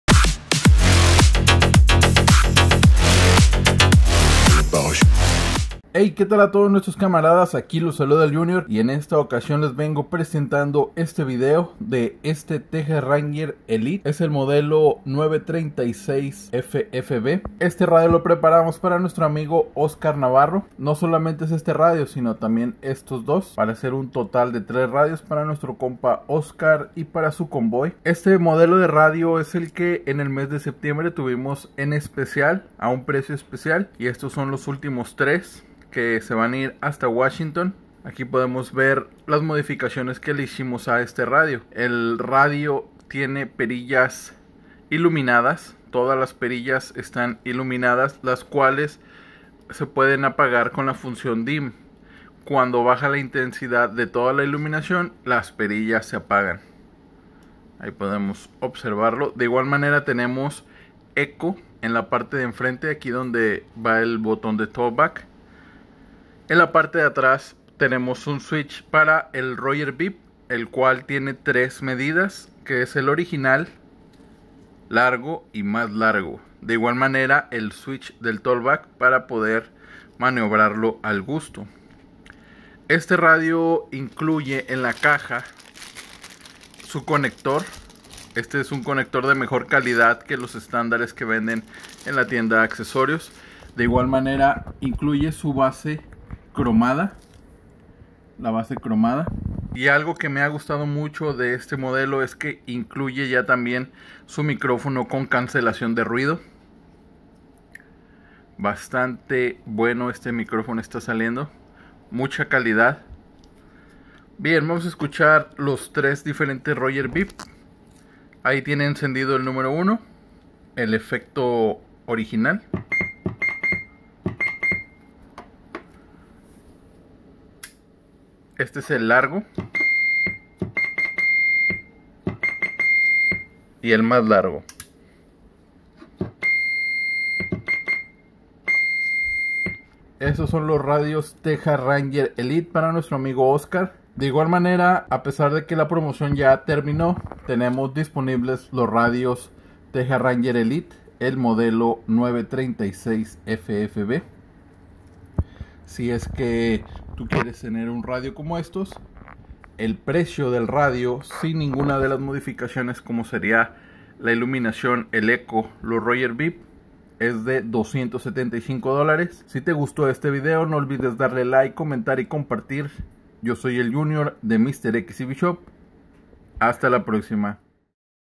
Texas Ranger Elite TRE-936FFB | MR.X CB SHOP
Estos son los radios Texas Ranger Elite para nuestro amigo Oscar espero que disfruten sus equipos, si tu quieres una radio como esta no dudes en poner en contacto con nosotros.